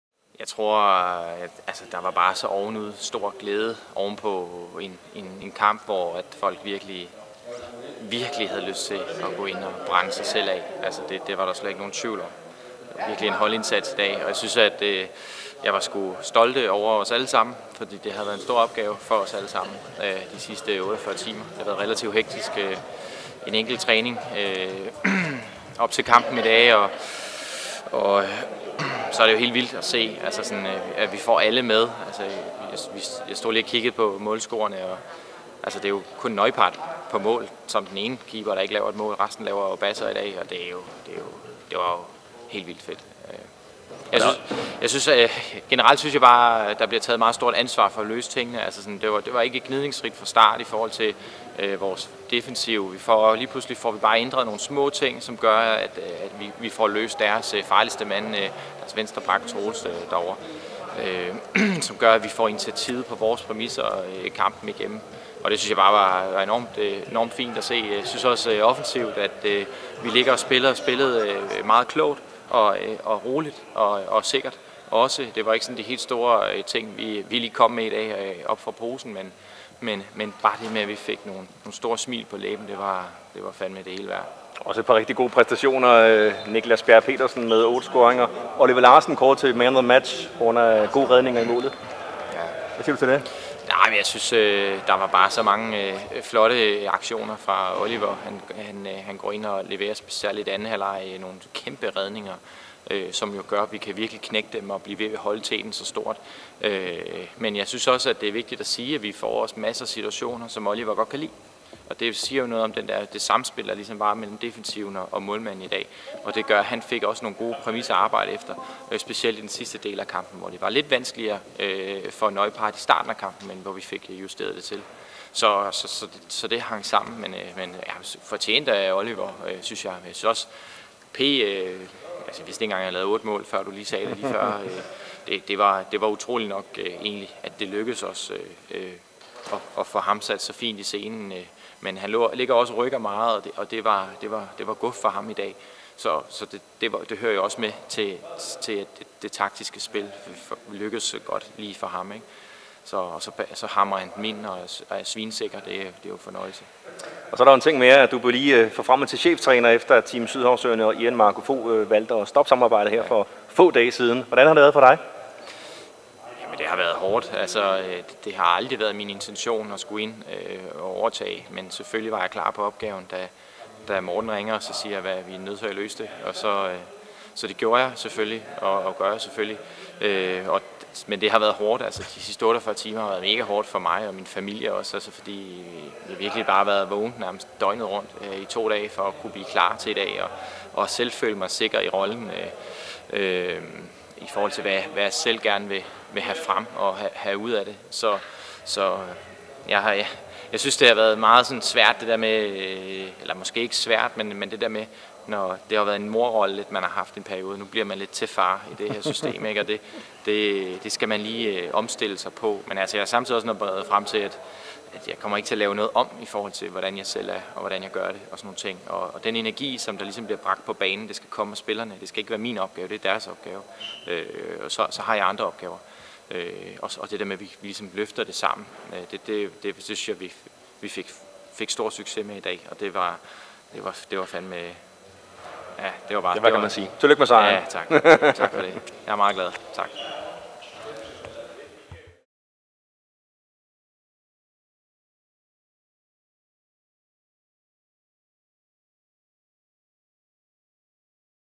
TSØ kørte Tønder over med 35-23
Team Sydhavsøerne havde ild i øjnene 🔥🔥 og kørte TM Tønder over med 35-23.
Den nye cheftræner Nick Szwiec fortæller her om kampen, og om hvordan de seneste par dage har været.